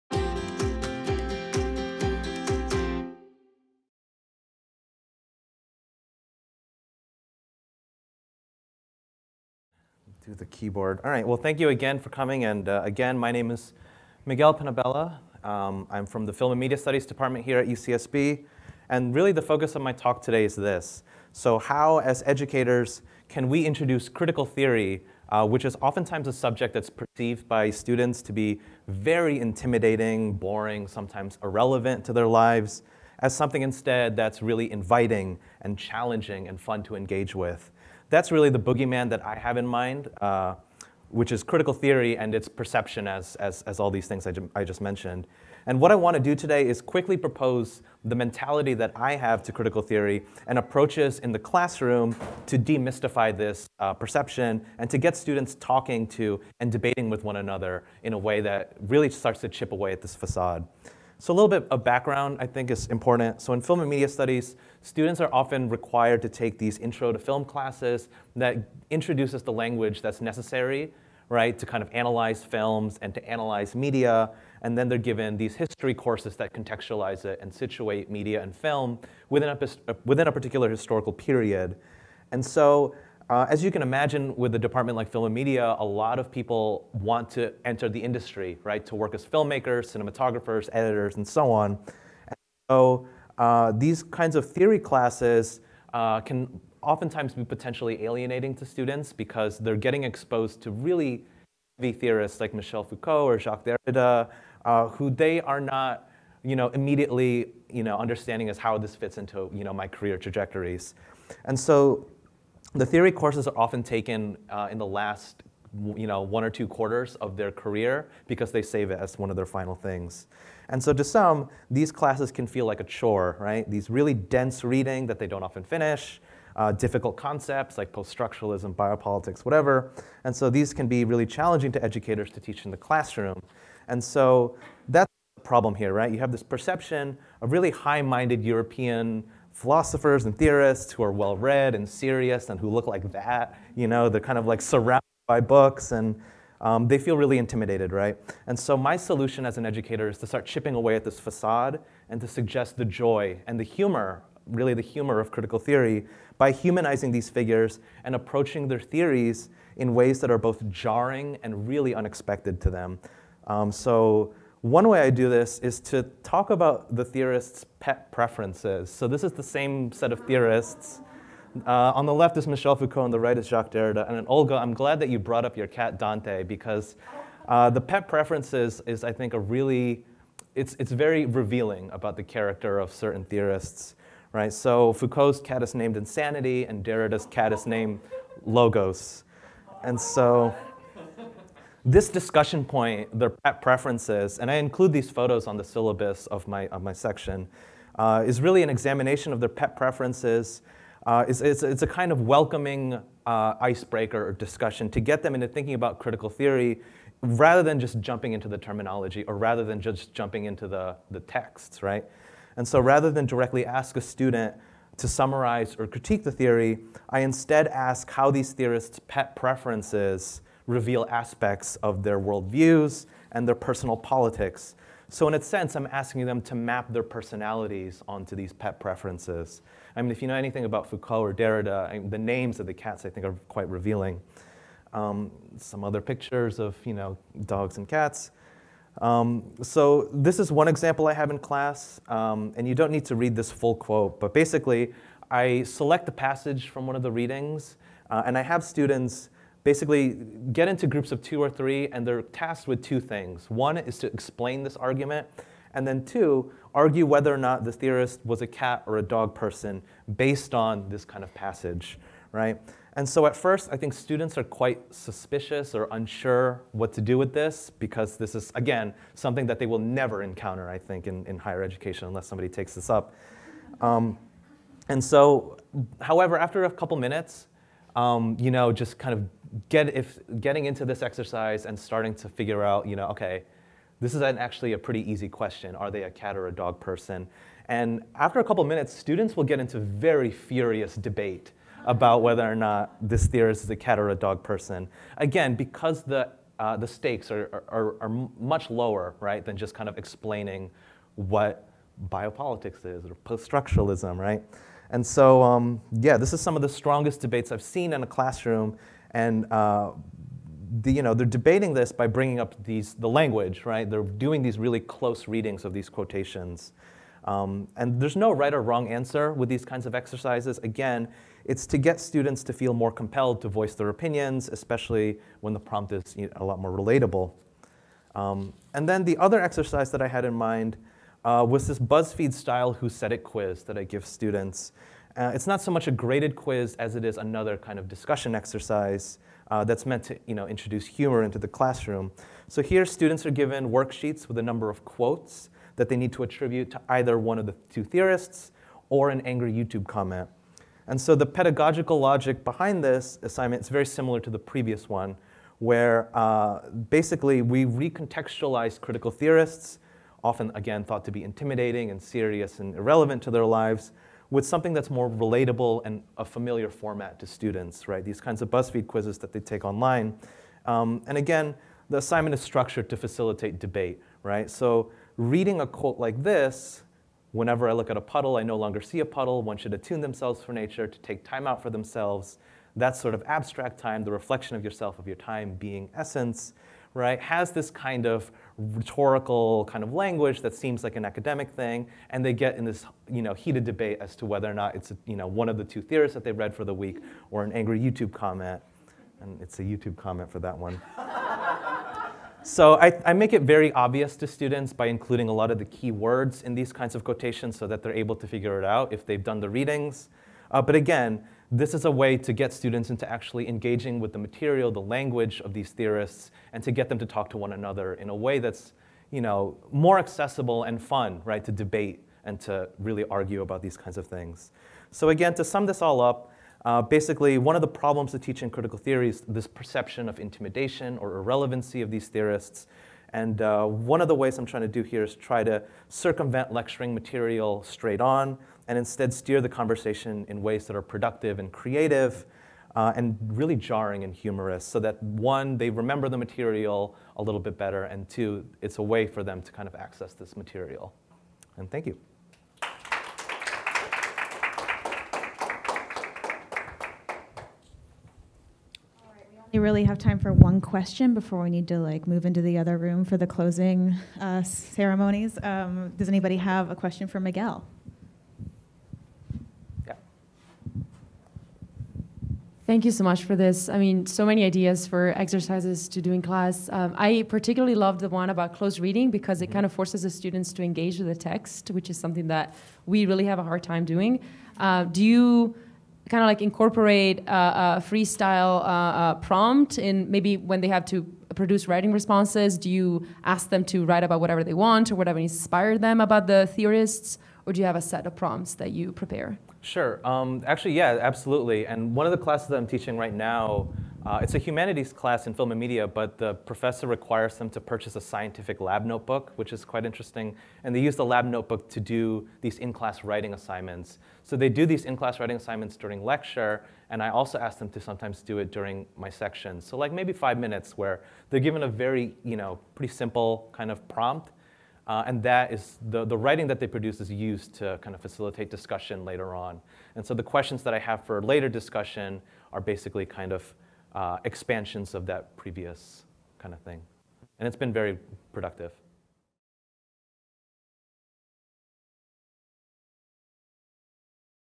[Miguel Penabella] Demystifying the Boogeyman Creative Engagements in Critical Theory default
Critical theory, while often dreaded by students as dry, boring, and intimidating to read, can be severed from such preconceptions with classroom activities designed to encourage creative thinking and humor. I will share my efforts in swapping lectures and discussions with activities like satirical BuzzFeed online quizzes and personality tests to teach Michel Foucault and Louis Althusser, catering to different types of learning and jolting students out of their scholarly boredom.